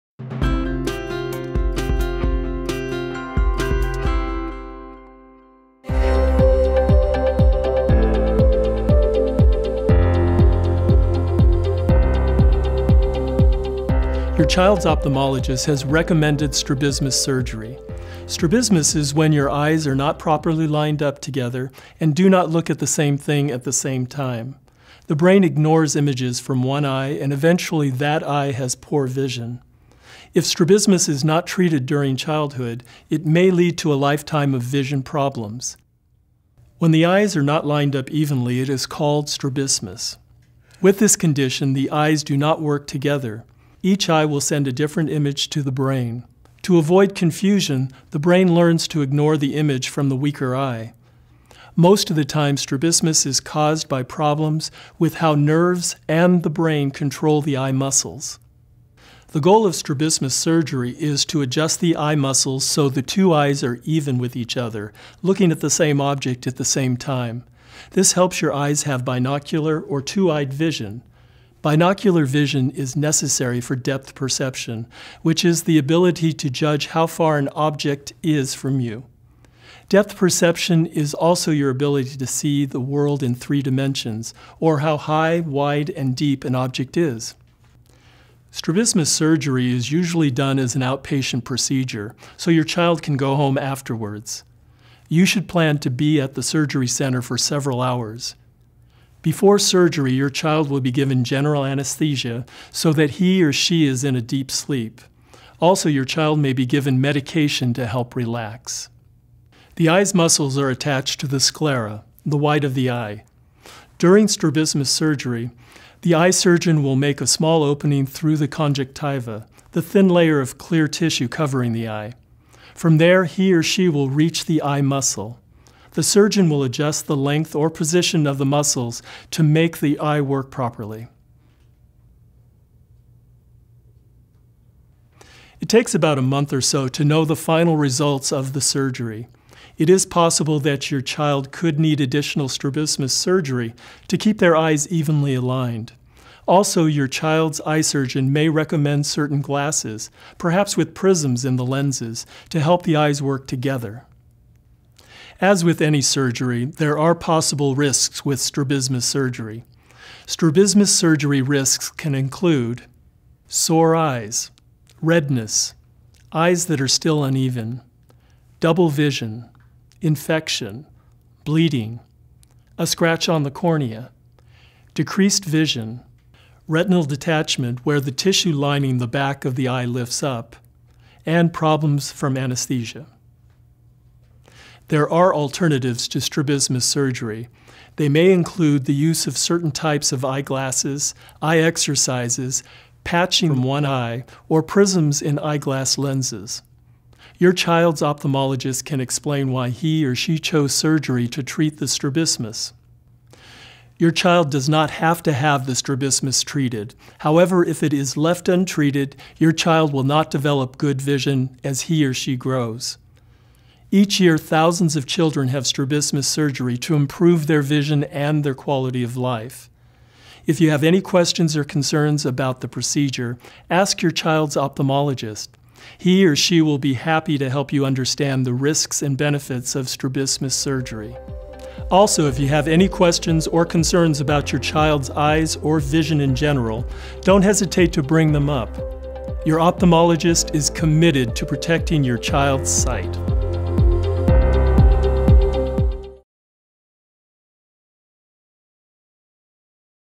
Strabismus Surgery Children
Strabismus is a common condition among children in which the eyes are not aligned properly and point in different directions. Learn more about this condition in this video.